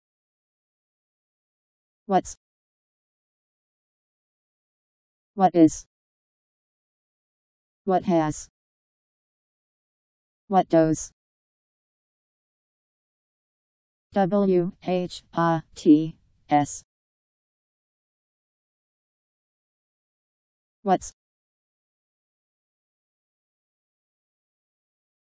What's Meaning
Video shows what what's means. What is.. What has.. What does..  what's pronunciation. How to pronounce, definition by Wiktionary dictionary. what's meaning. Powered by MaryTTS